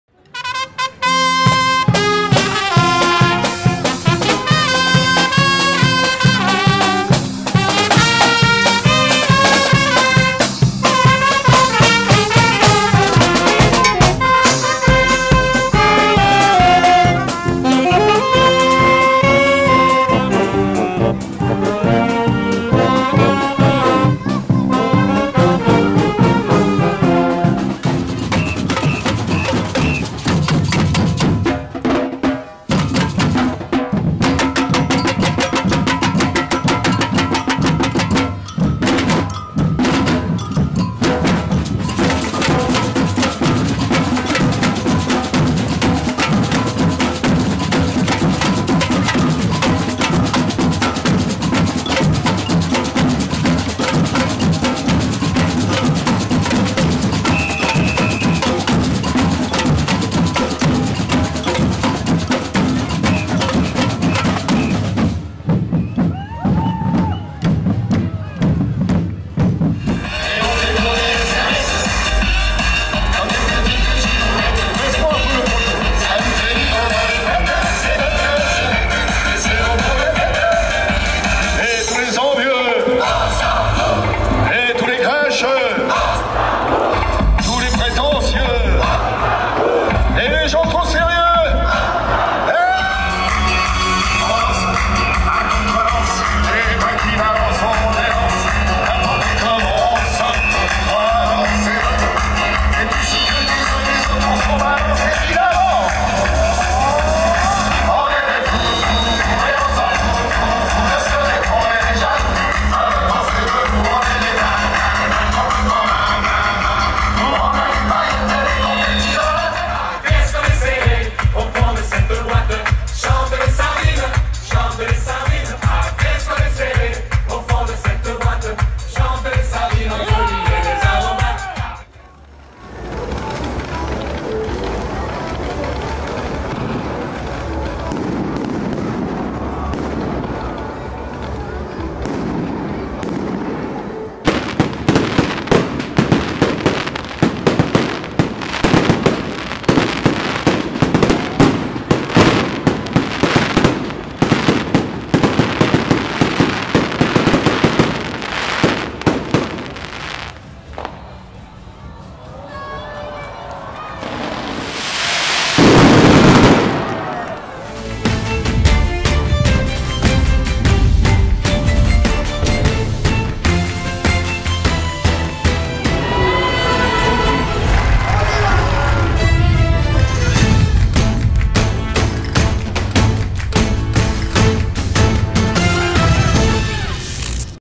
FERIA D'ALÈS 2023 - MERCREDI 17 MAI - Les festivités sont lancées !
Ce mercredi 17 mai, la 32e Feria de l'Ascension a débuté, rassemblant déjà plusieurs milliers de personnes dans le centre-ville d'Alès.

Les festivités ont officiellement été lancées lors du défilé inaugural, par le Max Roustan et Lysa Blanc-Nalle, Miss Alès 2023. Un parcours rythmé par les peña "Les Aux Temps Tics", "El Fuego", et "Los Caballeros" et suivi par la Manade Tommy Maire.

À 21h, le grand public a profité de la traditionnelle pégoulade, s'élançant de la place Gabriel Péri. L'ambiance était assurée par la batucada AJAIO, la batucada du Conservatoire à Rayonnement Intercommunal Maurice André d’Alès Agglomération, les Peñas "Les Aux Temps Tics", "El Fuego", les brésiliennes de Tubo Bem Bateria, ainsi que les manades Tommy Maire, Aubanel et Agnel.

La soirée d'ouverture s'est poursuivie à 22h sur la place des Martyrs-de-la-Résistance, où DJ Fanou et Patrick Sébastien ont enflammé la foule avec un showcase exceptionnel. Les plus grands titres de l'artiste populaire ont été repris en chœur par les Grand-Alésiens.

Le public a ensuite pu profiter d'un sublime feu d'artifice, tiré depuis le parvis du CinéPlanet.

pour découvrir le programme complet des animations qui se poursuivent jusqu'au dimanche 21 mai.

👉 sur Snapchat ➡ Ville d'Alès @villedales